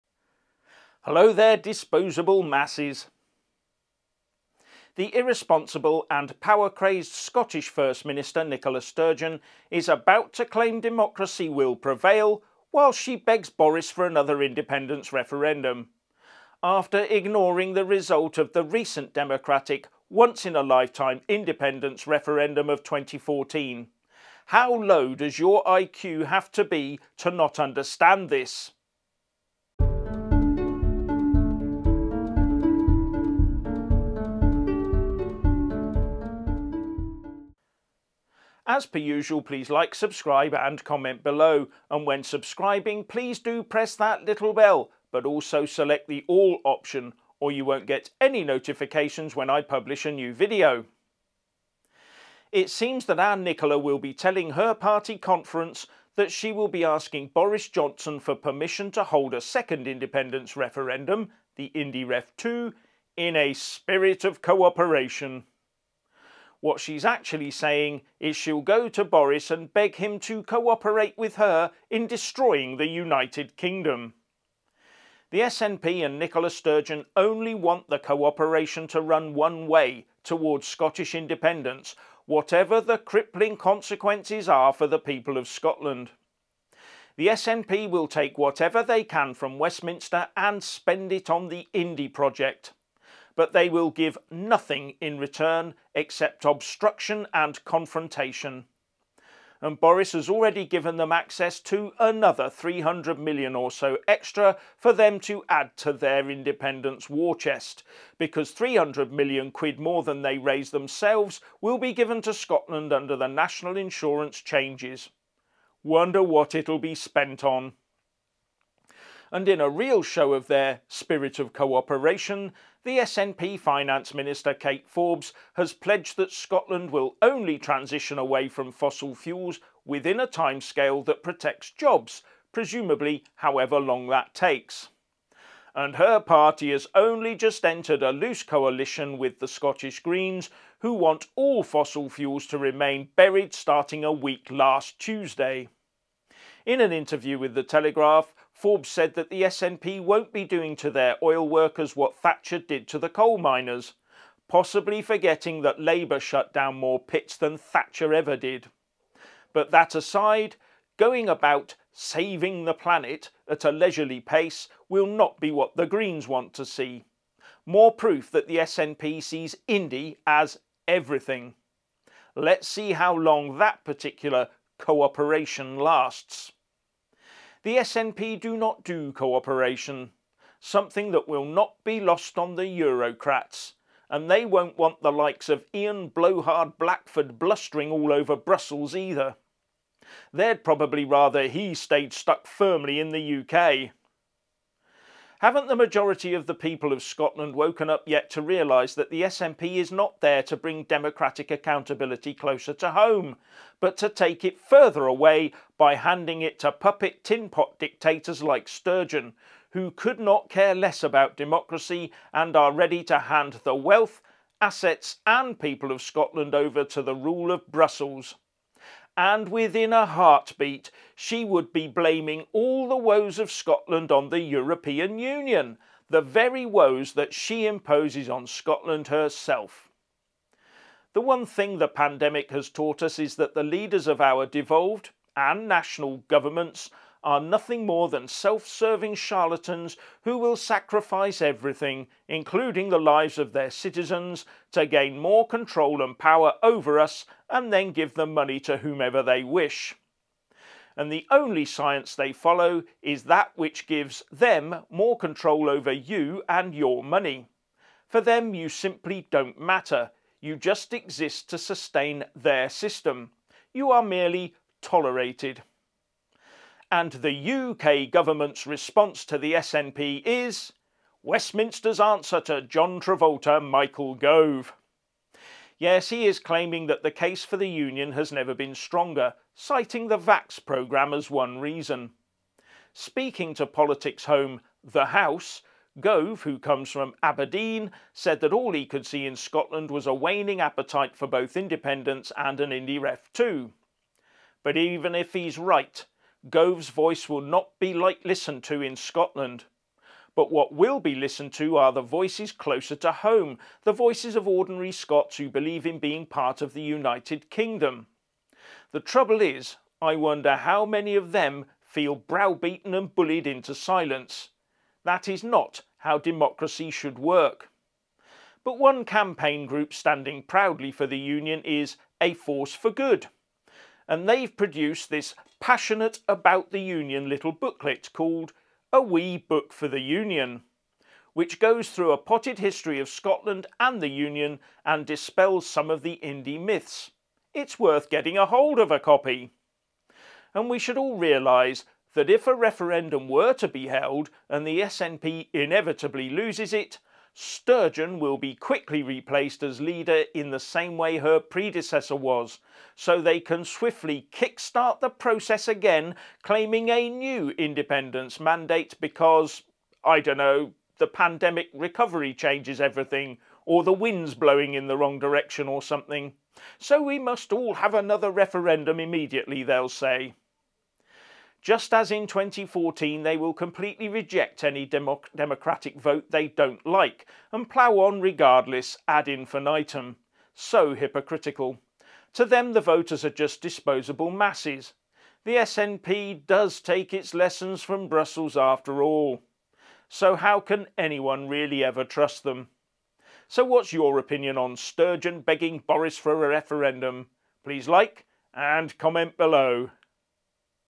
Sturgeon the hypocrite!
The irresponsible and power crazed Scottish First Minister, Nicola Sturgeon, is about to claim democracy will prevail whilst she begs Boris for another independence referendum.